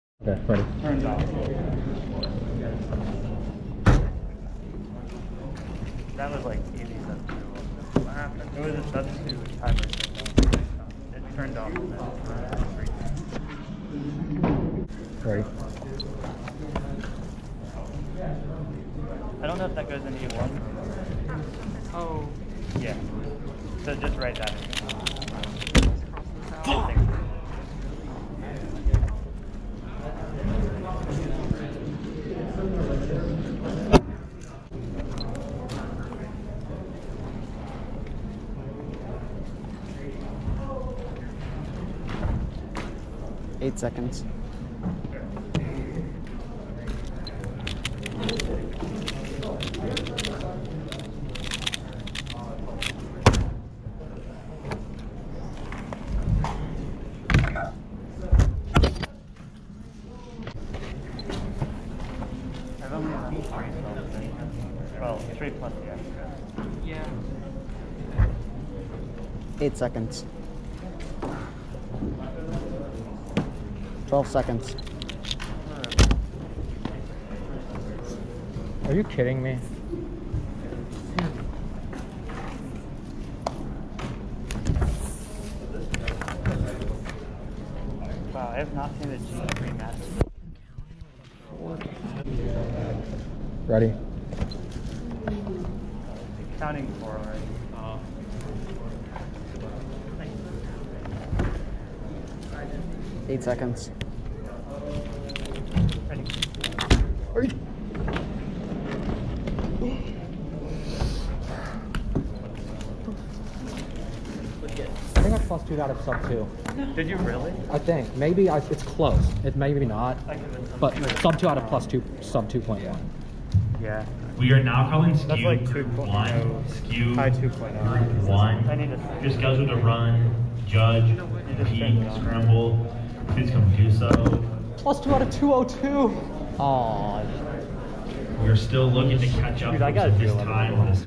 2.02 Pyraminx Dayton Record Average Fail
Getting close to three years without a sub 2.57…